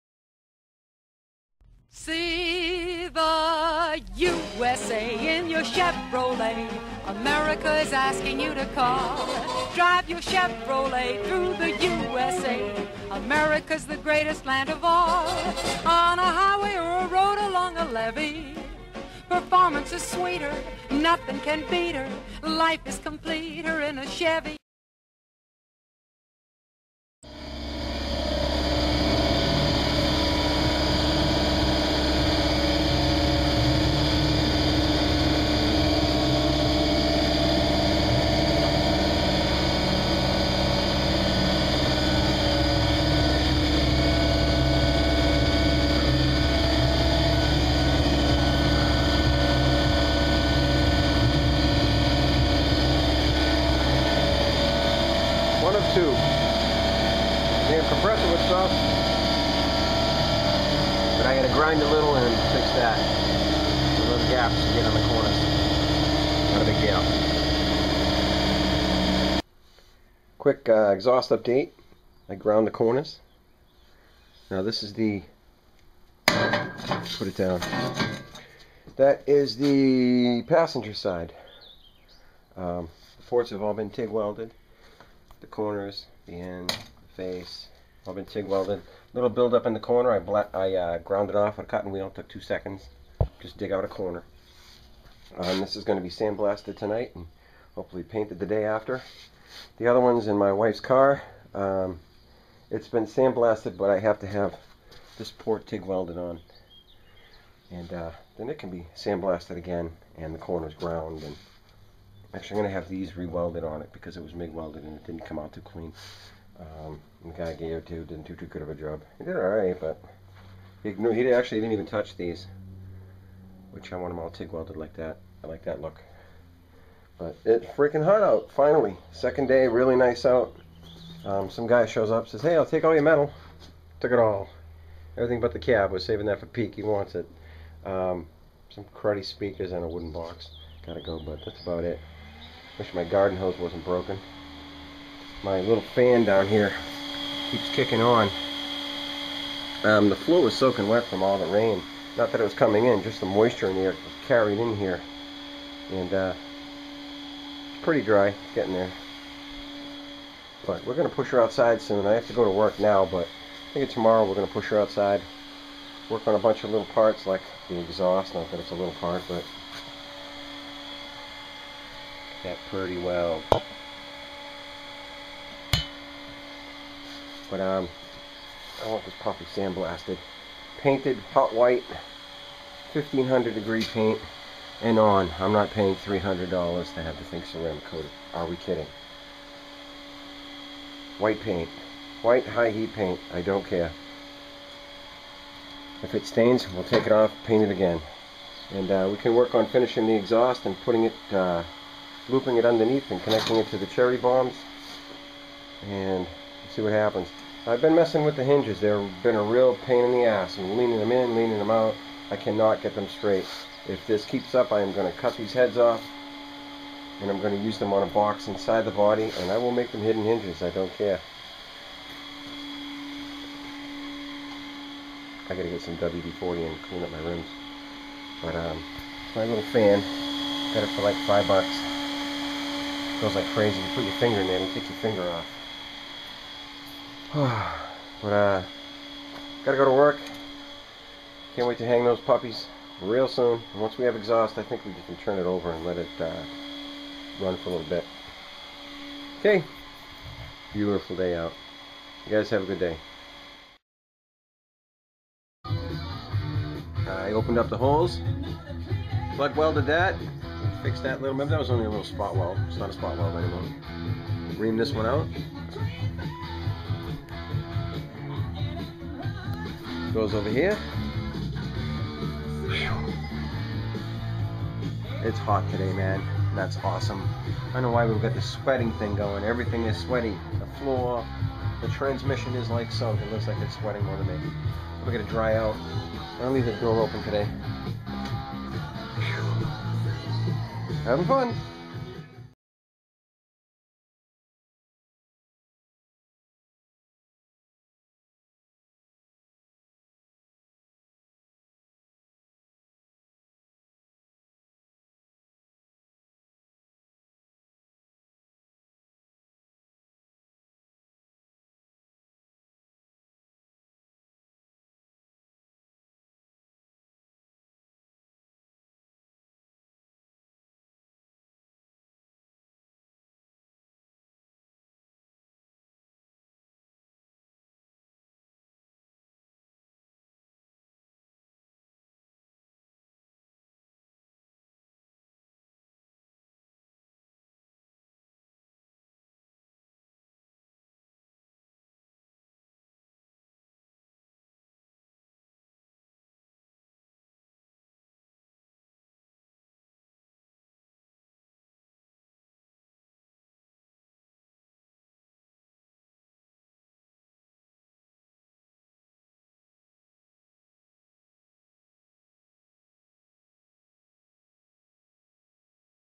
EVELYN #154 - Lake Pipes almost DONE & Hinge Adjustments
Just a few things to keep you updated.. ONE of the Headers is 99% complete and needs only blasting and paint.. the other awaits minor details and then it too is ready to go.. and a little on the Hinge area up front..